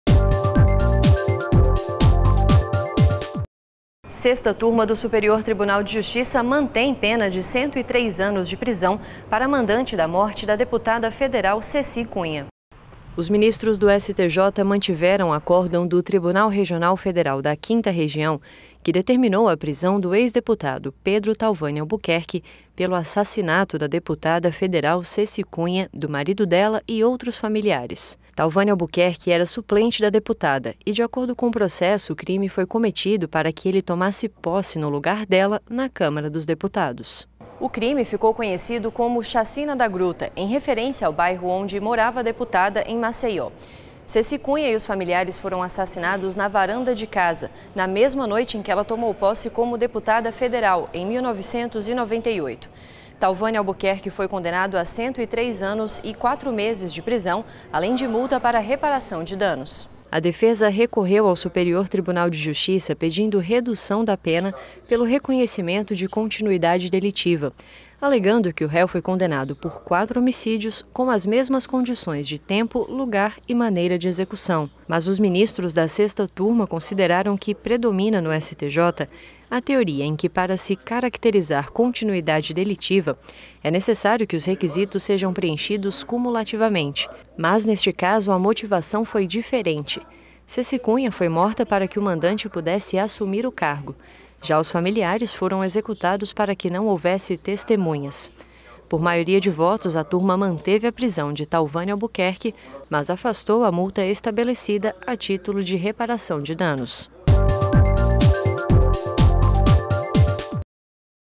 Mantida pena de 103 anos para mandante da morte da deputada federal Ceci Cunha (19.11.19)
​​A Sexta Turma do Superior Tribunal de Justiça confirmou acórdão do Tribunal Regional Federal da 5ª Região que manteve a pena de prisão imposta ao ex-deputado federal Pedro Talvane Albuquerque pelo assassinato da deputada federal Ceci Cunha e de outros três integrantes de sua família, afastando apenas a multa estabelecida a título de reparação de danos.